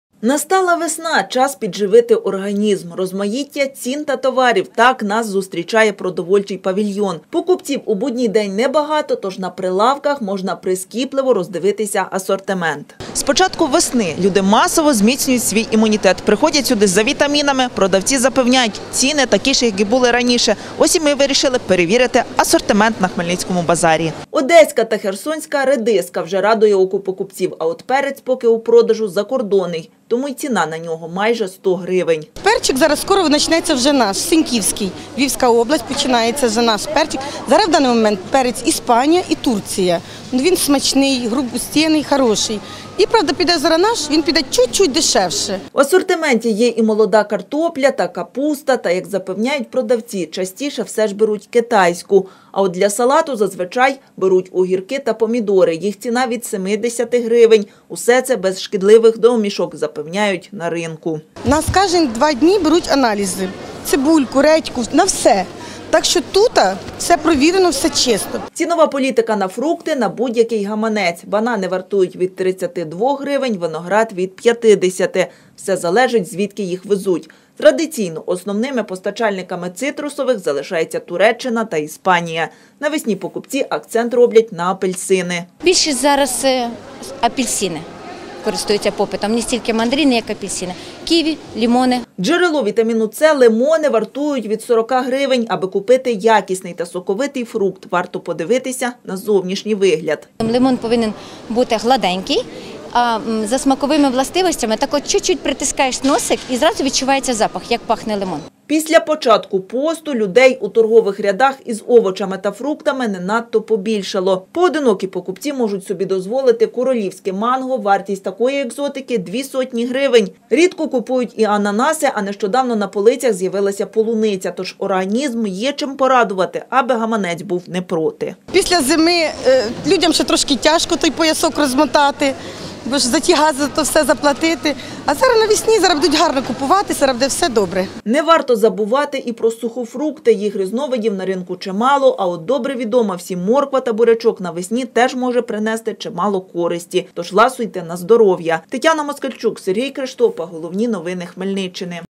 Поповнити організм вітамінами – яка цінова політика на хмельницькому ринку ?
Від екзотики до повсякденних овочів – що можна купити на хмельницькому ринку, аби поповнити запас вітамінів ?